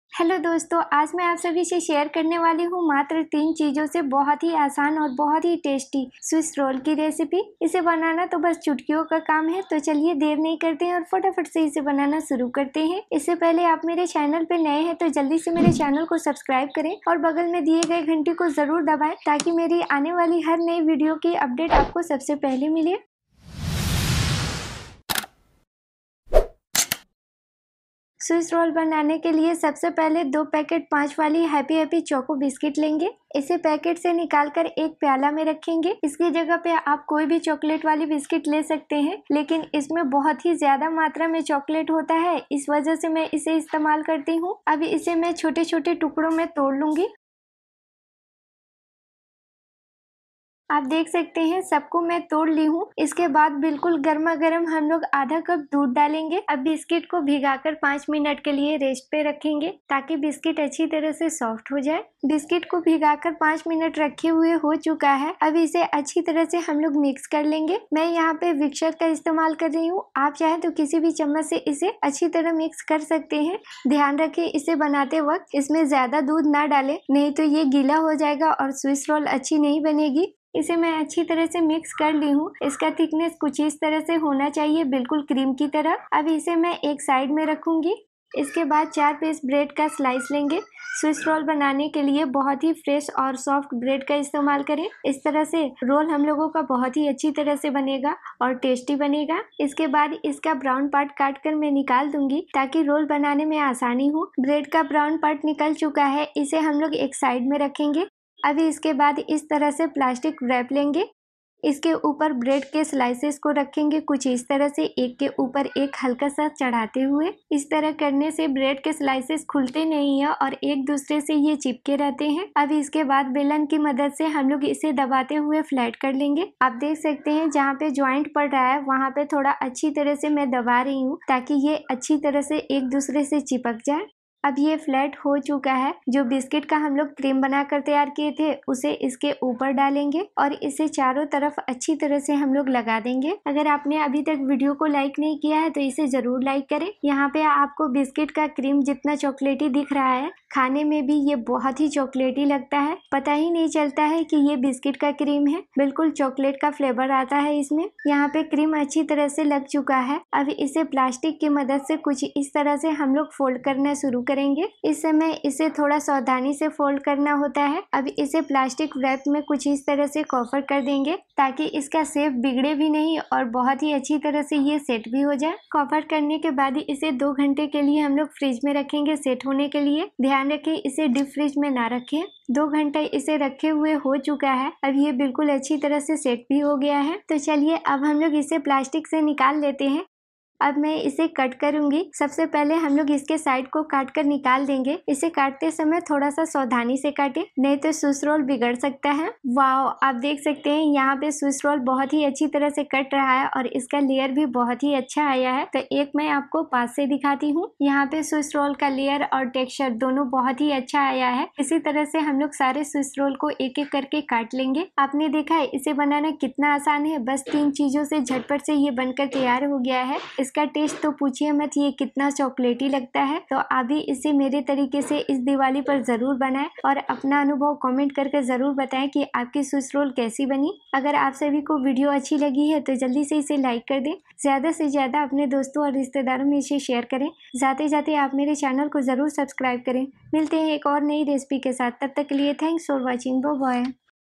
Chocolate Swiss Roll | Diwali Mithai | Diwali Sweets Recipe
Hello Everyone Today I am going to share Diwali Special Easy Chocolate Swiss Roll Recipe. Hope you will try it at your home and share your experience with me in the Comment section.

00:00 This Chocolate swiss roll is very easy to prepare at home - Introduction
00:27 How to prepare Chocolate Cream
01:28 Most Important Part Don't Skip it
03:00 Final